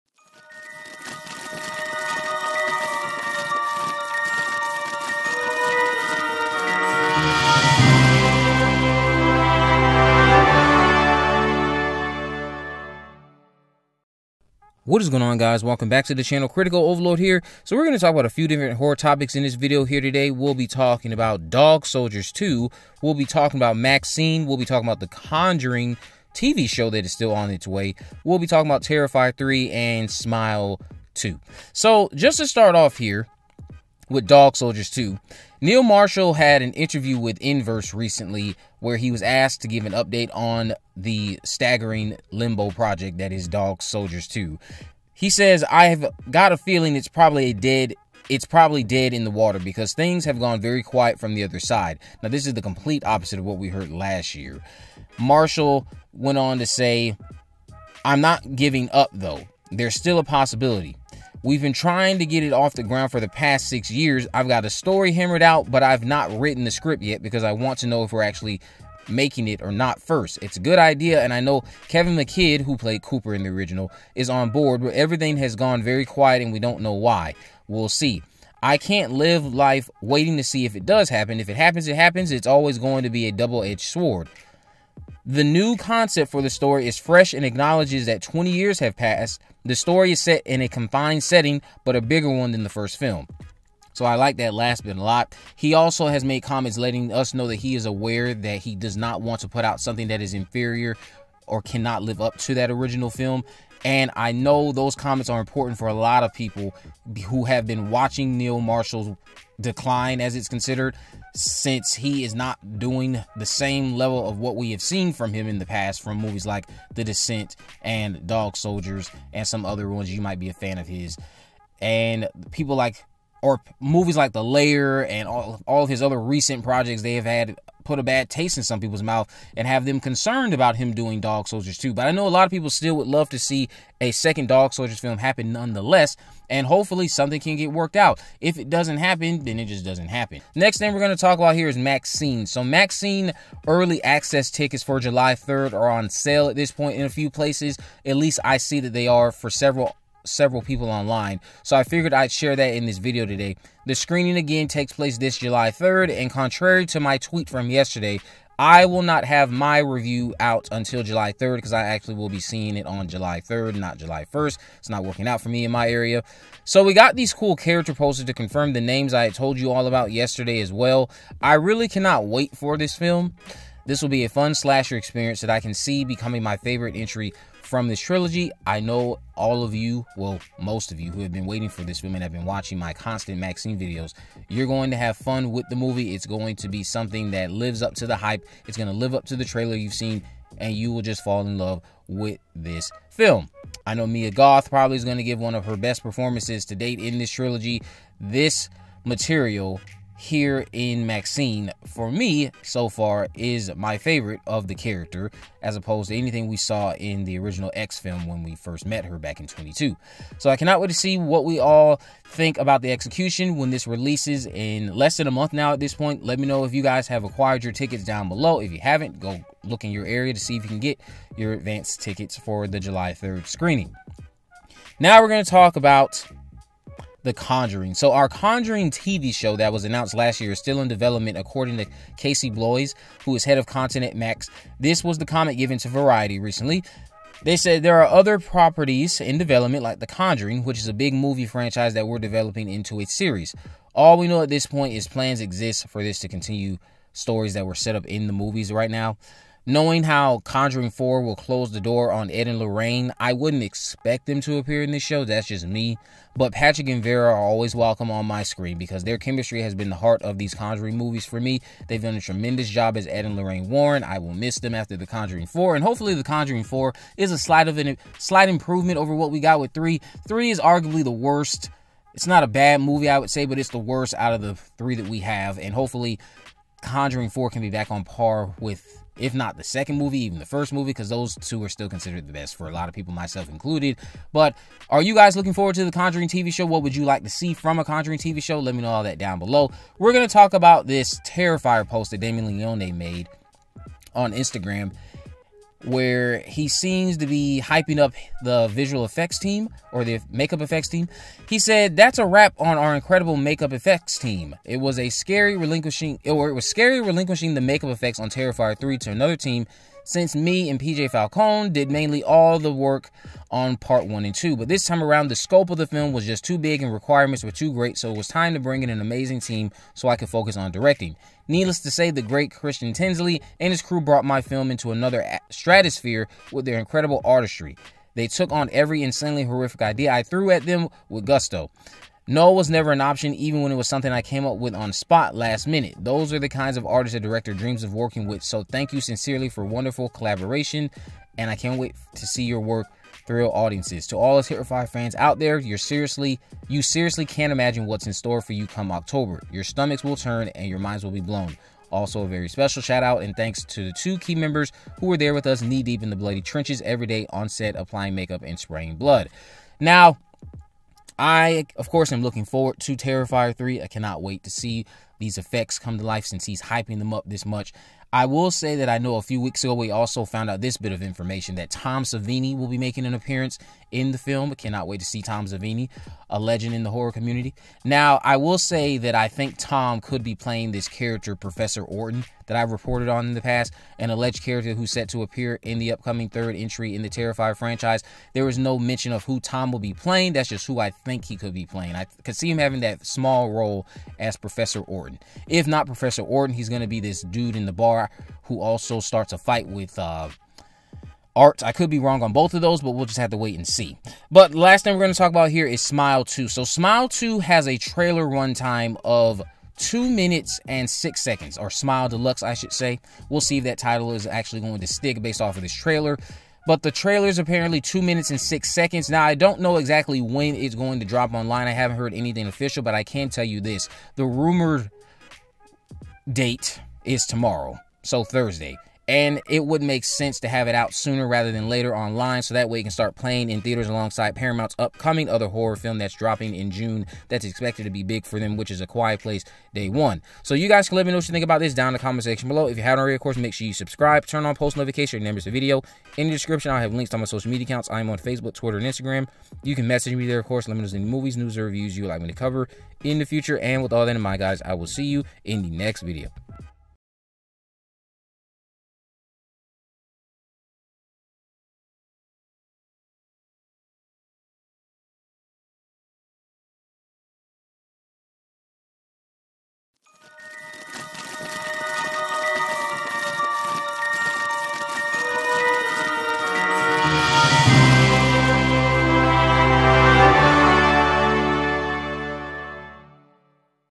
SMILE 2 Trailer, MAXXXINE News, The Conjuring Show, Terrifier 3 Update, Dog Soldiers 2 Plot
Smile 2 trailer could arrive online this week since two are reportedly ready for release. The Conjuring tv show from Max is still in development according to Max content head. Terrifier 3 will feature Tom Savini in an undisclosed role, and more. MAXXXINE posters have confirmed the character names I revealed. Dog Soldiers 2 is still struggling, but we have an update.

0:00 Intro
0:14 Dog Soldiers 2
2:20 MAXXXINE
3:44 The Conjuring Show
4:50 Terrifier 3
6:45 SMILE 2
7:50 Outro